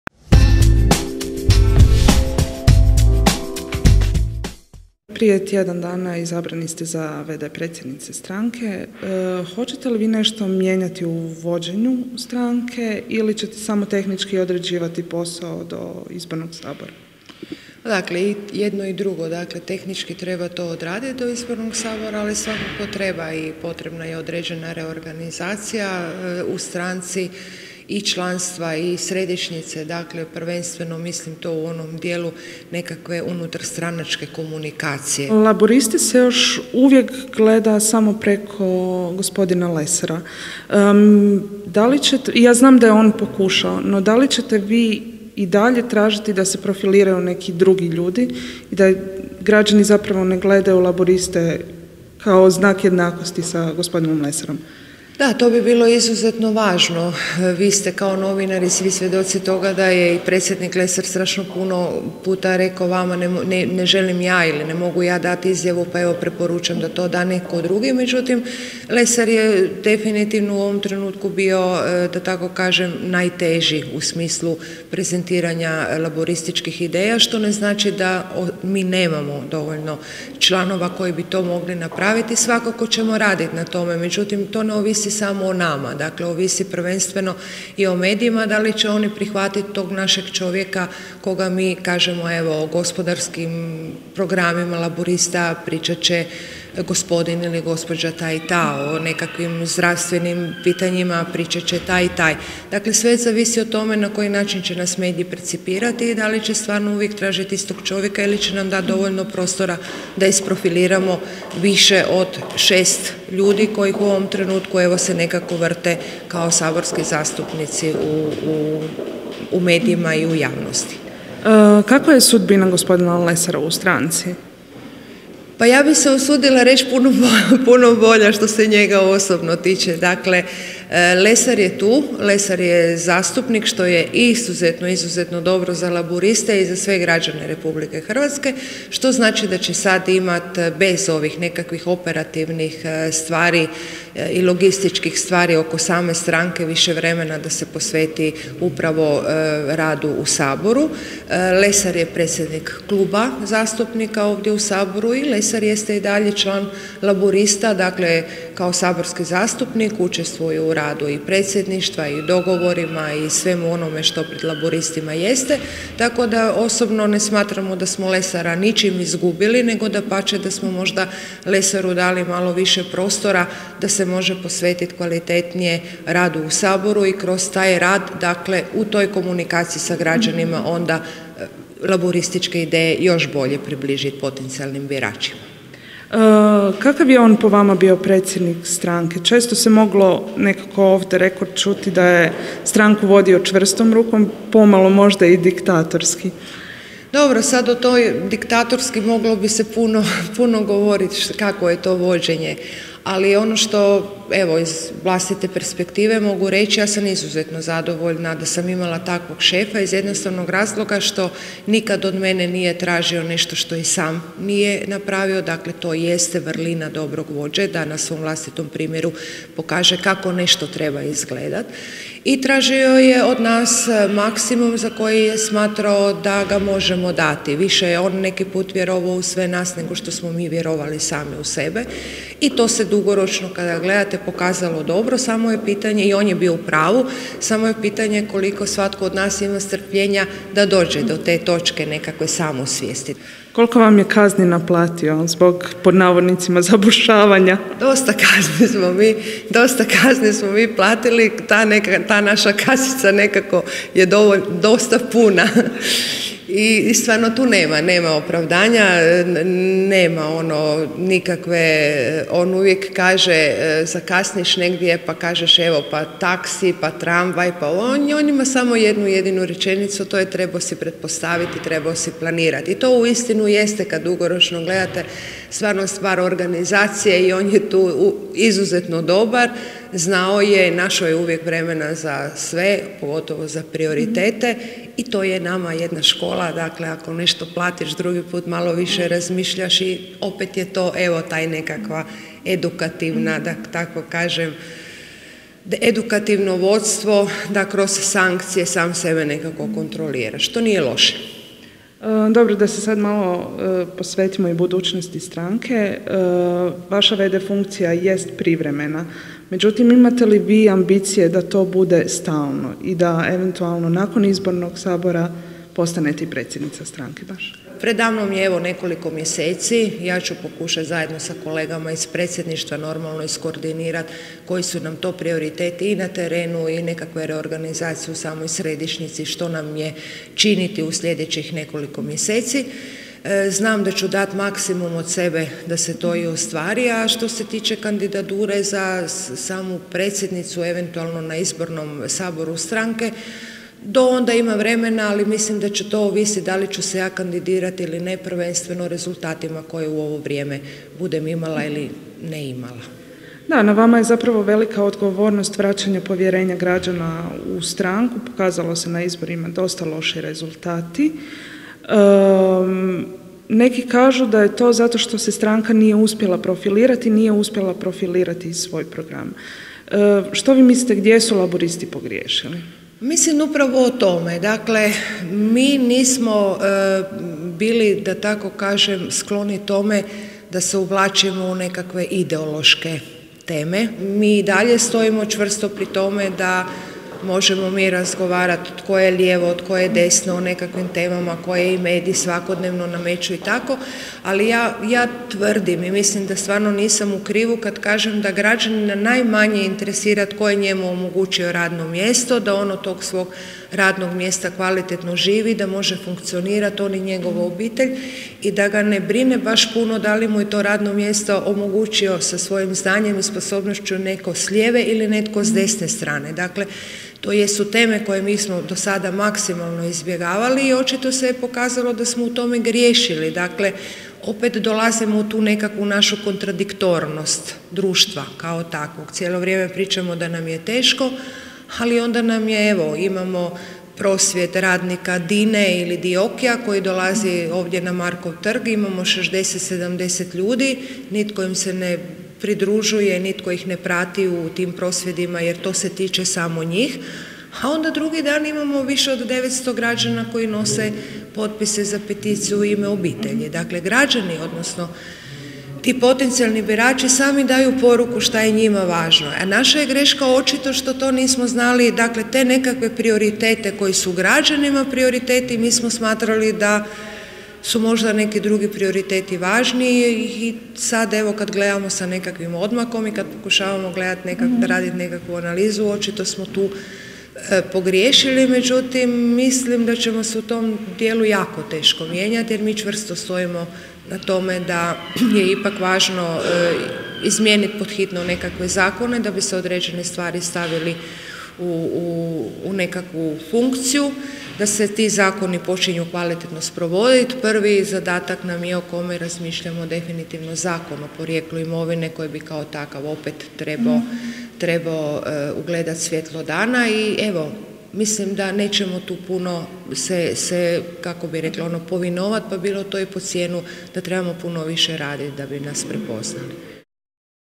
Laburisti | Nansi Tireli otkriva sudbinu Hrvatskih laburista i Dragutina Lesara
Mogu li li se Laburisti, nakon izbornog poraza, vratiti među glavne igrače na političkoj sceni, kakva je sudbina dosadašnjeg predsjednika stranke Dragutina Lesara, te ima li ambicija naslijediti njega na čelnom mjestu u prvom dijelu intervjua za tportal pokušala je odgovoriti saborska zastupnica Hrvatskih laburista Nansi Tireli

Vlasnik priloga: Tportal